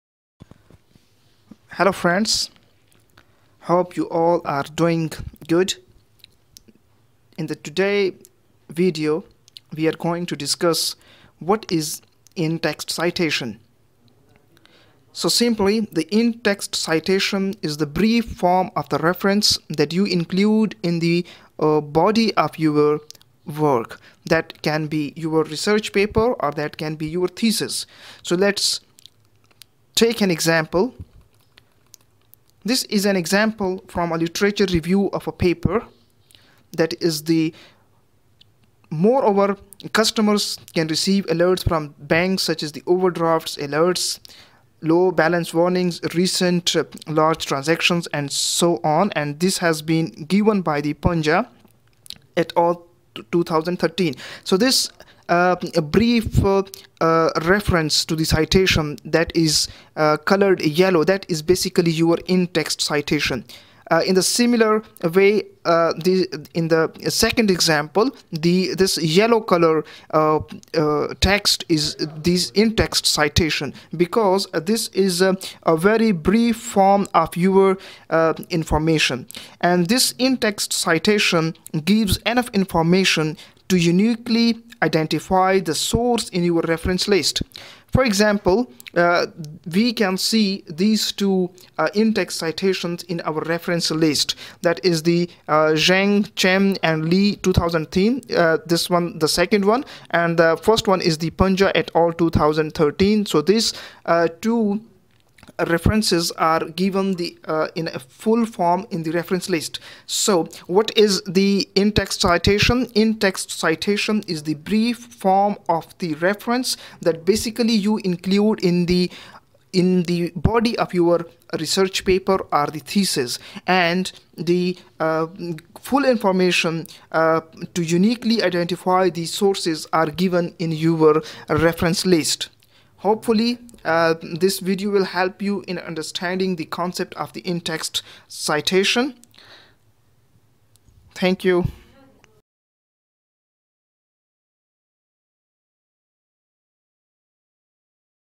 what are In-Text Citation and Referencing || In-Text Citation in APA format
In-Text Citation and Referencing || In-Text Citation in APA format ||

In text citation is the brief form of the reference that you include in the body of your work.

In-TextCitation